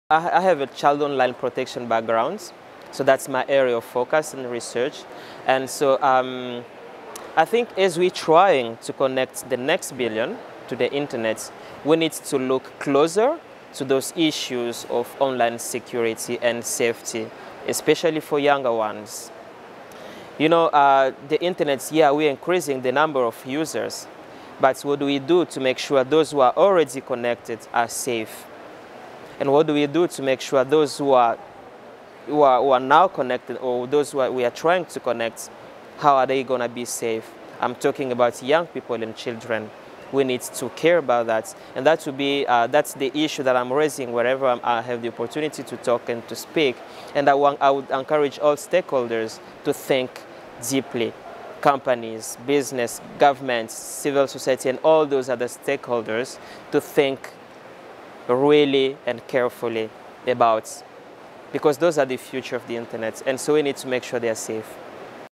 Global IGF 2015: Arsene Tungali Baguma on Top Internet Issues
Arsene Tungali Baguma, member of the Internet Society Ambassadors Program, shares his answer to the question, “What are the most important issues that must be addressed in order to assure the best future for the Internet and all people?” Recorded at the Global Internet Governance Forum 2015 in João Pessoa, Brazil, by the Imagining the Internet Center, an initiative of Elon University, North Carolina, USA.